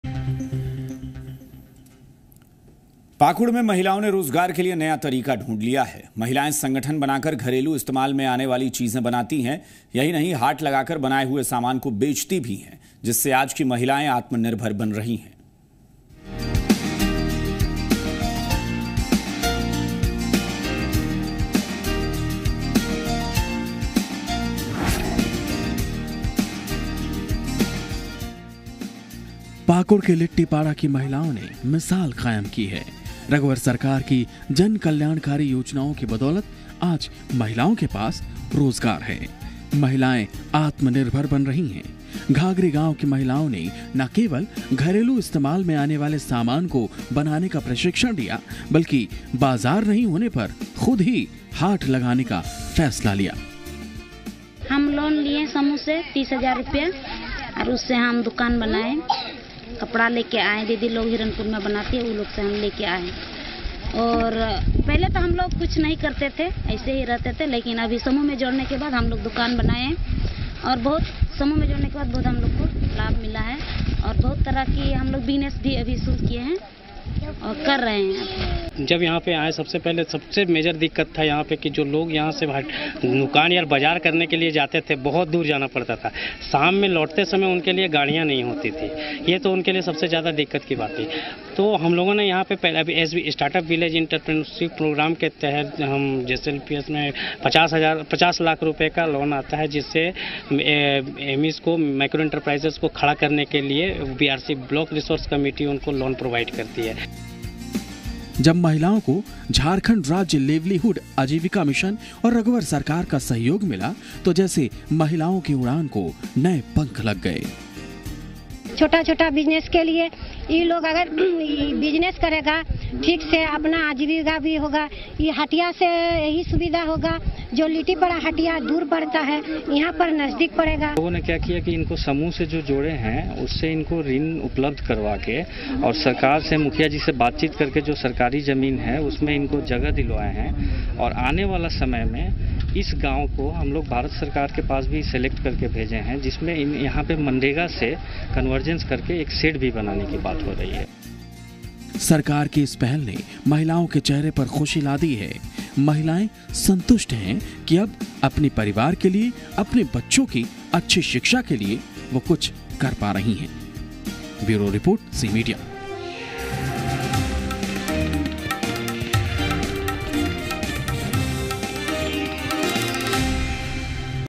उन्नत झारखंड: पाकुड़ में महिलाओं ने रोजगार के लिए नया तरीका ढूंढा।। Unnat Jharkhand
पाकुड़ में महिलाओं ने रोजगार के लिए नया तरीका ढूंढ लिया है...महिलाए संगठन बनाकर घरेलू इस्तेमाल में आने वाली चीज़े बनाती है .यही नहीं हाट लगाकर बनाए हुए सामान को बेचती भी हैं. जिससे आज की महिलाएं आत्मनिर्भर बन रही हैं.